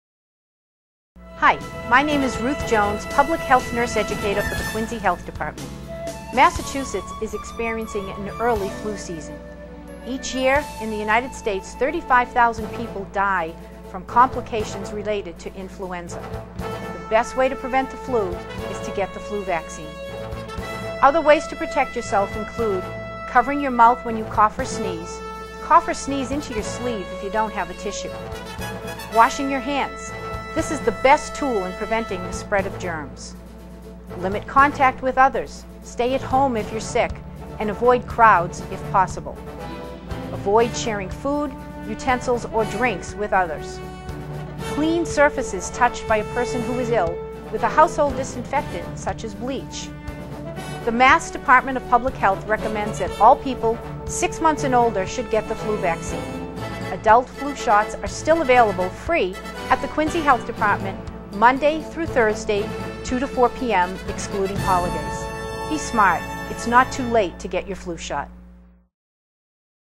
Quincy Health Department Flu PSA
Ruth Jones, Public Health Nurse Educator for the Quincy Health Department, talks about the early flu season and steps to take to prevent contracting the flu.